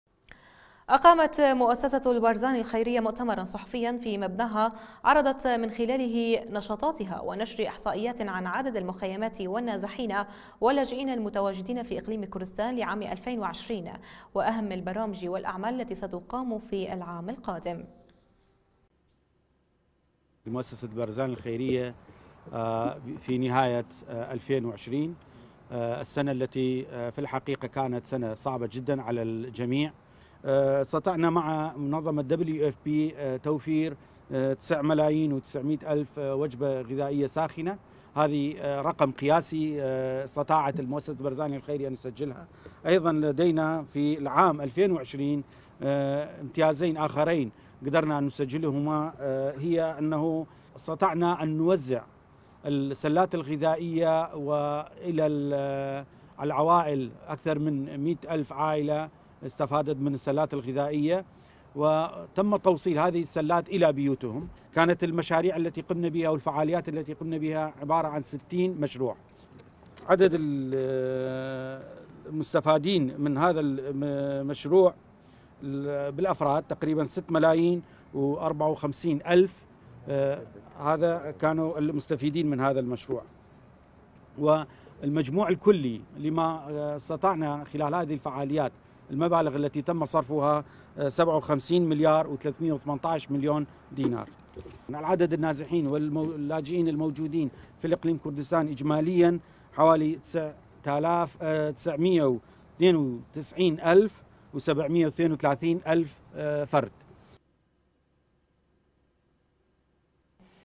استعراض النشاطات للعام 2020 واهم البرامج والاعمال للعام 2021
مؤسسة البارزاني الخيرية

أقامت مؤسسة البارزاني الخيرية مؤتمرا صحفيا في مبناها عرضت من خلاله نشاطاتها ونشر إحصائيات عن عدد المخيمات والنازحين واللاجئين المتواجدين في اقليم كردستان لعام ألفين وعشرين واهم البرامج والأعمال التي ستقام في العام القادم